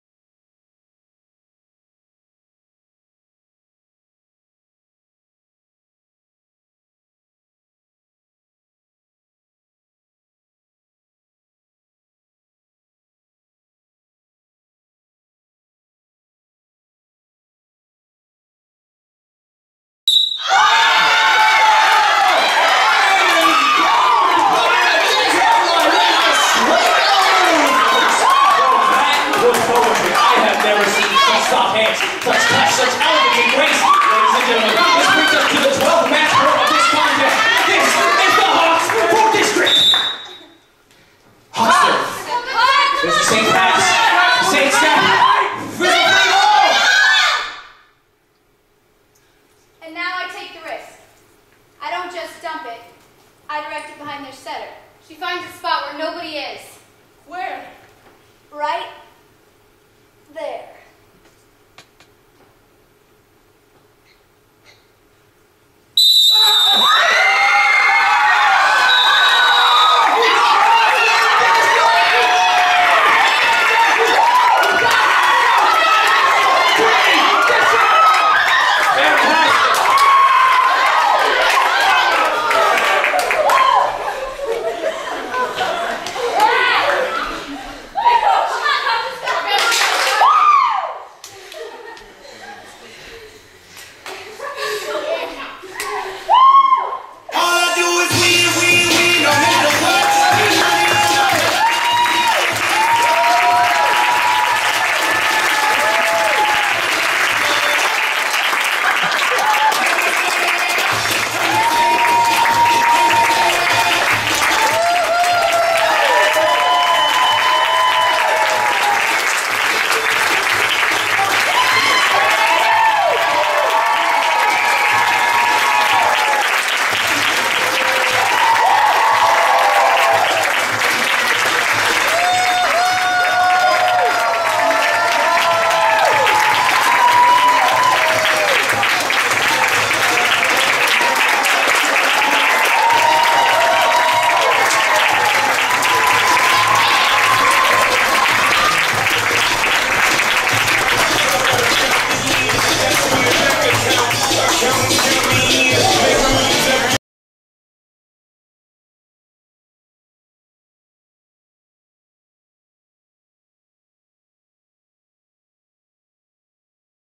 Volleygirls by Rob Ackerman - Short - Curtain Call
Directed by T.J. Walsh
Scenic and Costume Design by Angelina Vyushkova
Lighting Design by Michael Skinner
Sound Design by Chris Hassler
Volleyball Choreography by Lyndsie Gregorie
Stage Manager: Claire Carden

Coach Jim Brindell. Gabriel Whitehurst
Dirk, Angry Dad. Craig Robertson
Jess, Captain and Setter. Kathleen Flowers
Ingrid, Middle Blocker. Sarah Sampson
Liv, Outside Hitter. Jenna Wiles
Marisol, Middle Hitter. Kelsey Milbourn
Crash, Rightside Hitter. Delaney Milbourn
Katie, Outside Hitter. Shelby Ringdahl
Jocelyn, Defensive Specialist. Caroline Iliff
Xavier, Sportscaster from brother school. Mitchell Stephens
Vlad, Referee. Scott Moffitt
Sally, Athletic Director. Christina Chester
Carla, Jess's mom. Annie Burton
Phil, Jess's dad. Matt Morris
Lauren, Katie's mom. Kim Borge
Ben, Crash's dad. Dylan Glaser
Hays Theatre, TCU